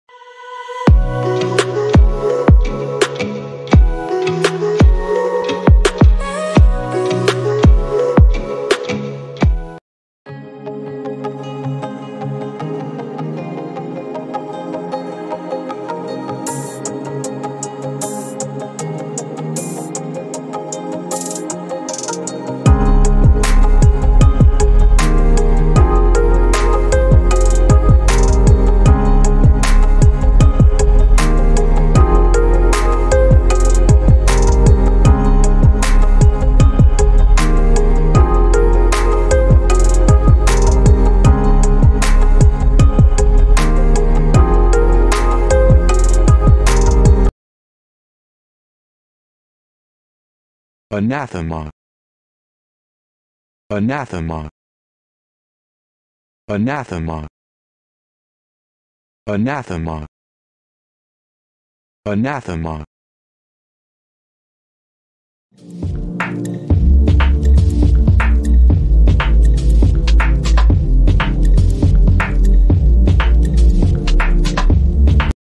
Anathema meaning, sentence and pronunciation
in this video u can learn
pronunciation, meaning of Anathema

Please support and love.

create account with my broker.

** PREMIUM TRAINING *

** PREMIUM TRAINING **